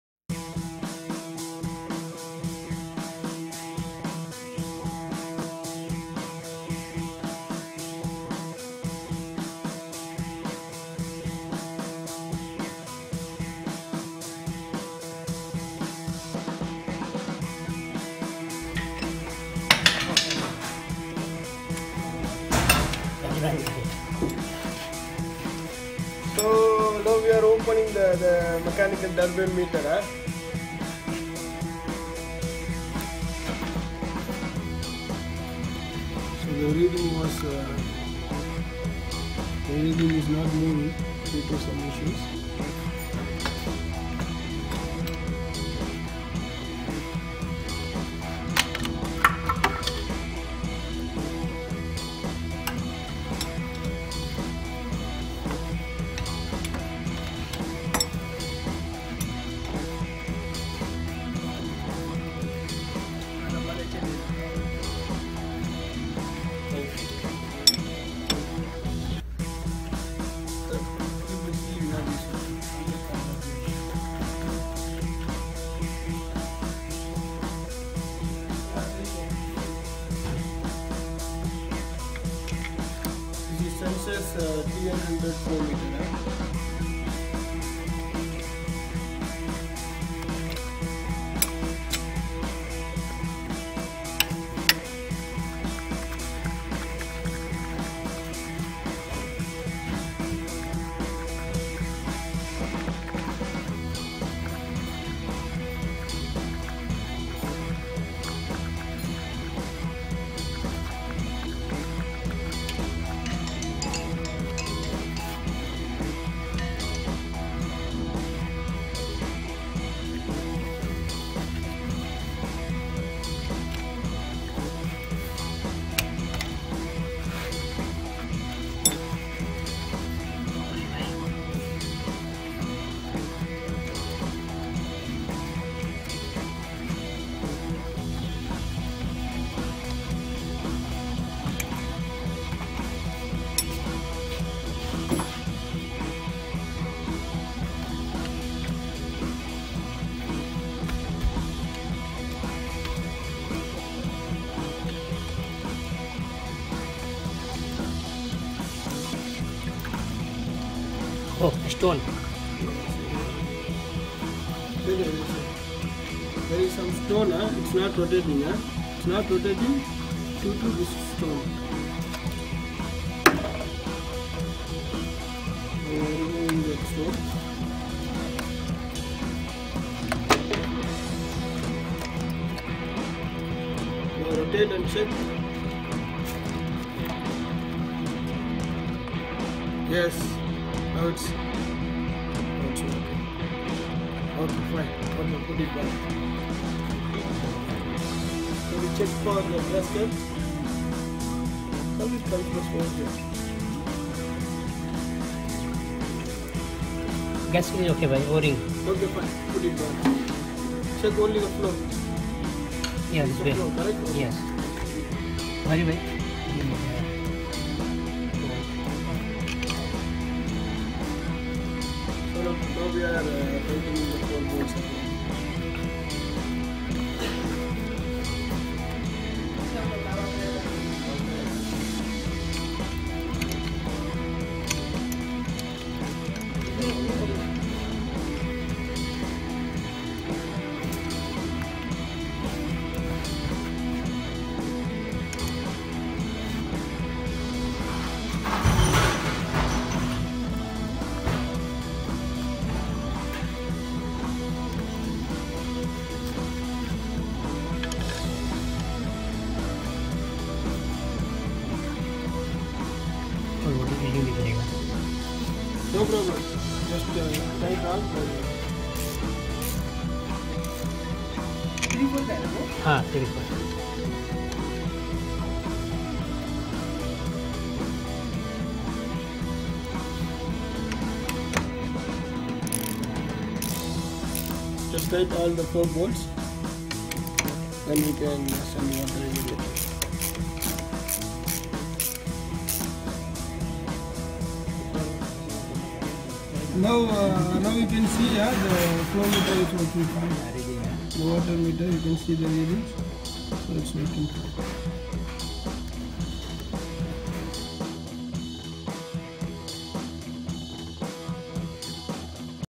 SENSUS WP DYNAMICS - WATER MECHANICAL TURBINE FLOW METER REPAIR WORK - Real Field Experience
Hi, I created this videos to show how to repair the water mechanical turbine flow meter along with how to assemble and disassemble them.

Thanks
Mech Plaza aka பட்டறை